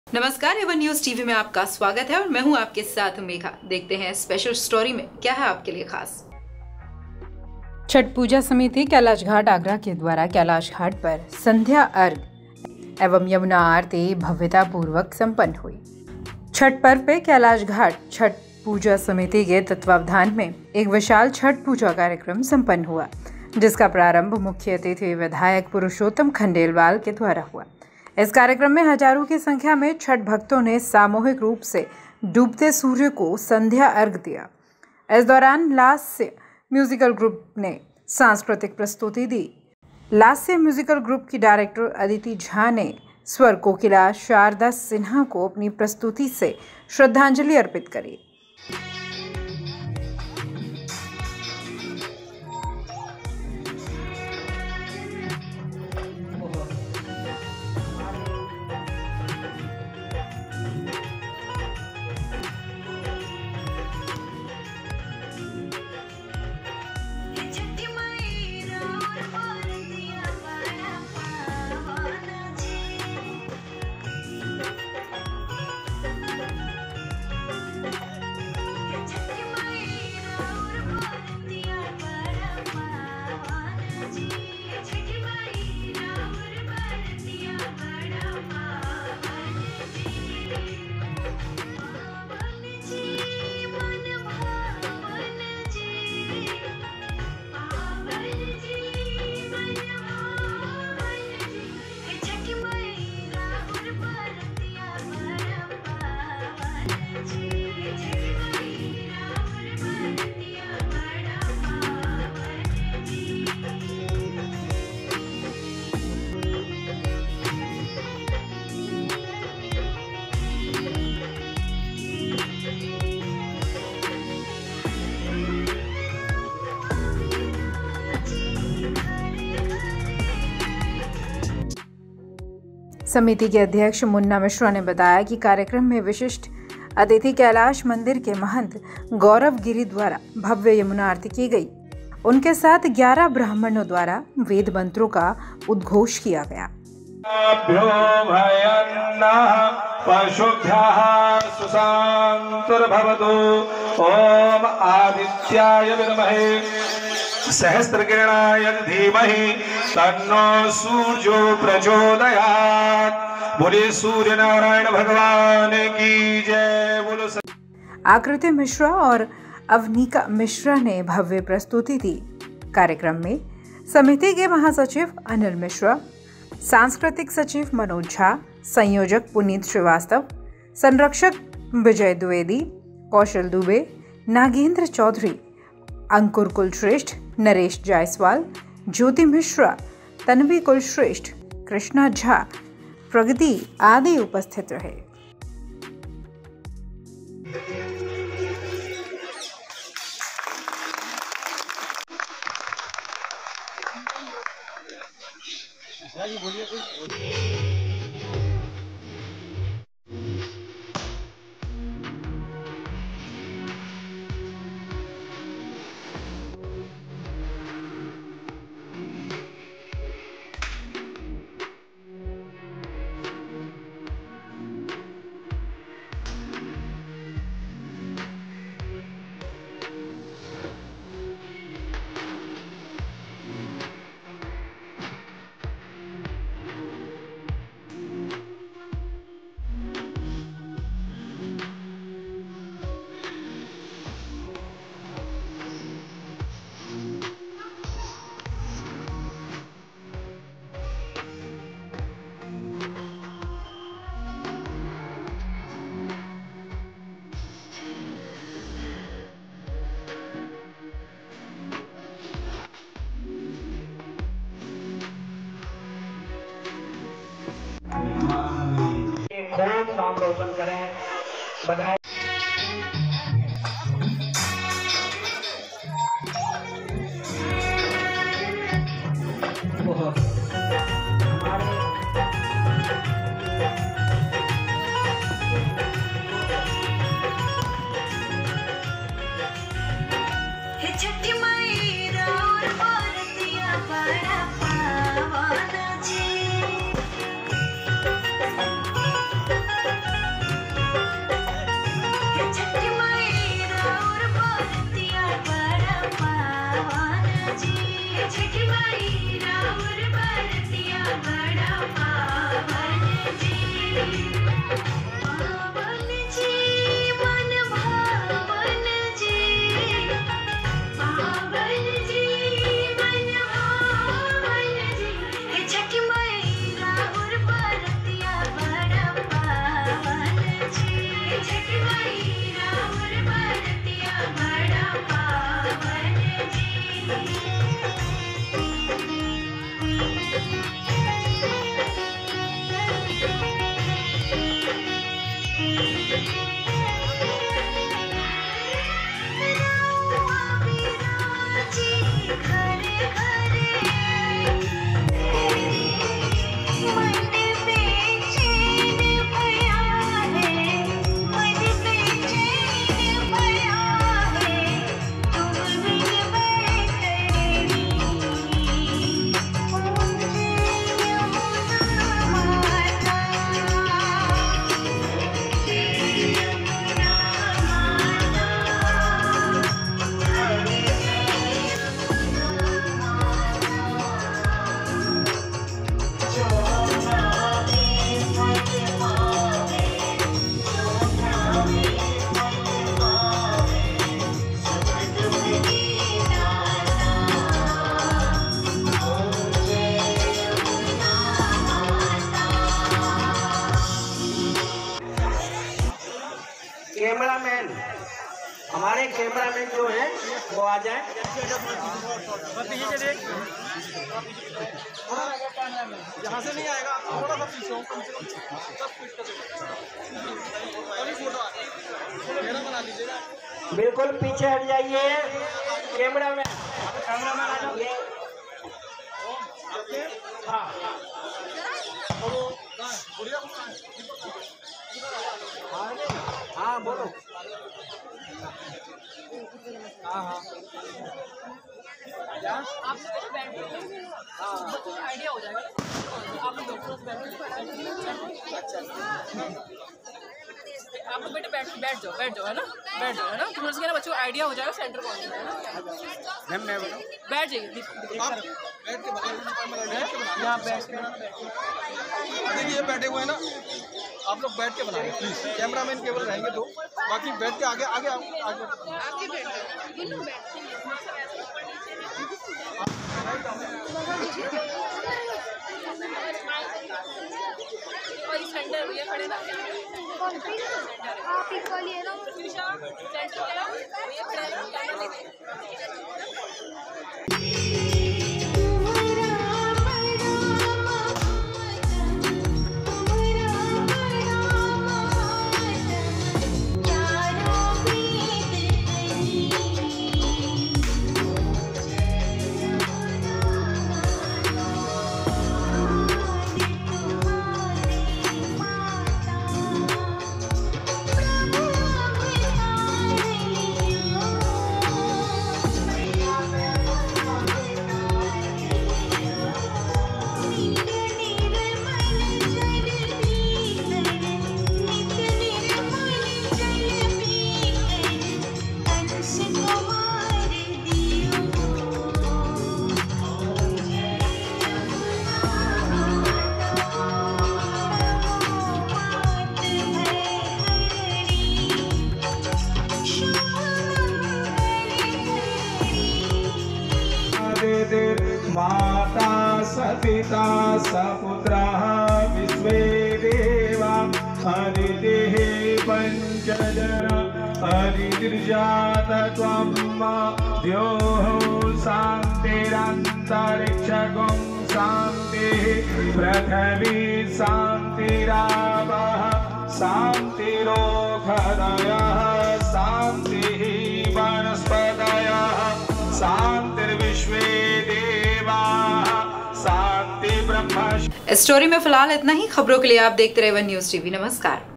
छठ पूजा समिति कैलाशघाट आगरा के द्वारा कैलाश घाट पर संध्या अर्घ्य-यमुना आरती संपन्न हुई | aone news
छठ पूजा समिति कैलाशघाट आगरा ने कैलाश घाट पर संध्या अर्घ्य एवं यमुना आरती  संपन्न की | aone news

आगरा: आज छठ पर्व पर कैलाश घाट छठ पूजा समिति के तत्वाधान में एक विशाल छठ पूजा कार्यक्रम सम्पन्न हुआ जिसका प्रारंभ मुख्य अतिथि विधायक पुरुषोत्तम खंडेलवाल जी के द्वारा हुआ। इस कार्यक्रम में हजारों की संख्या में छठ भक्तों ने सामूहिक रूप से डूबते सूर्य को संध्या अर्घ्य दिया।  इस दौरान लास्य म्यूजिकल ग्रुप द्वारा सांस्कृतिक प्रस्तुति दी गई। लास्य म्यूजिकल ग्रुप की डायरेक्टर अदिति झा ने स्वर कोकिला शारदा सिन्हा जी को अपनी प्रस्तुति से श्रद्धांजलि अर्पित की। समिति के अध्यक्ष मुन्ना मिश्रा ने बताया कि कार्यक्रम में विशिष्ट अतिथि कैलाश मंदिर के महंत गौरव गिरी  जी द्वारा भव्य यमुना आरती की गई। उनके साथ 11 ब्राह्मणों द्वारा वेद मंत्रों का उद्घोष किया गया। आकृति मिश्रा और अवनीका मिश्रा द्वारा भव्य प्रस्तुति दी गई। कार्यक्रम में समिति के महासचिव अनिल मिश्रा सांस्कृतिक सचिव मनोज झा संयोजक पुनीत श्रीवास्तव संरक्षक विजय द्विवेदी कौशल दुबे, नागेंद्र चौधरी अंकुर कुलश्रेष्ठ, नरेश जायसवाल, ज्योति मिश्रा तन्वी कुलश्रेष्ठ कृष्णा झा, प्रगति आदि रहे।